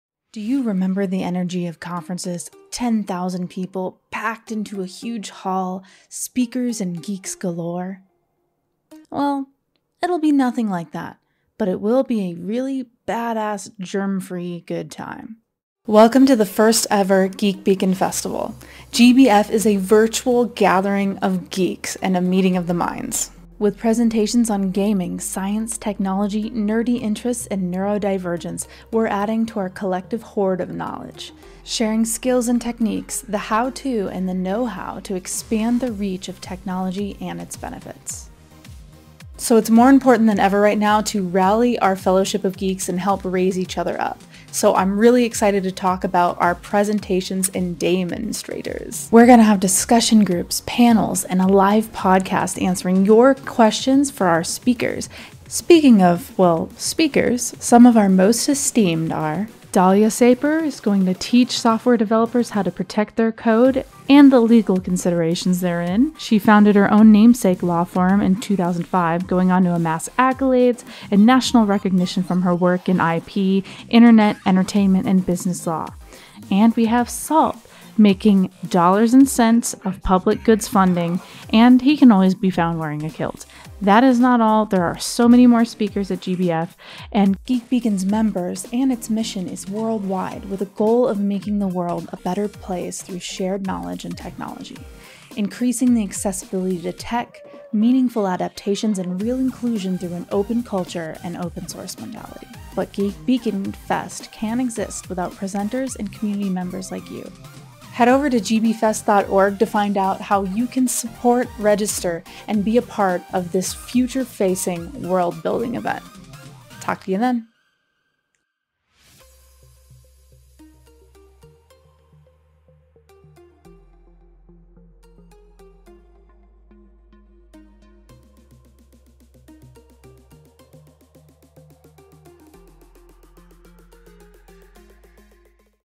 Join our Free Epic Open Source Geek Culture Conference opensource foss 2/18 - 2/19 2022
There will be talks on science, technology, and how it can improve our lives. New hosts announced @SoldierKnowsBest, @dom& @ShinyTechThings! We’ll discuss OSS projects and gaming, showcasing the myriad applications and benefits of both.

In short, GeekBeacon Festival is an open culture, open source festival with the goal of making the world a better place through shared knowledge and technology. Join us on February 18th & 19th, 2022, from anywhere with an internet connection as we celebrate those values!

GeekBeacon Festival is a conference and showcase in which we share information, techniques, knowledge and skills with one another to grow a collective focused on outreach and inclusivity. It’s more important than ever to make connections to build networks of mutual support and enrichment during these times, to expand accessibility through innovative adaptations.

At GeekBeacon Fest we will be exploring the unique value and experiences of neurodivergent individuals and the ways they thrive as vital members of our society.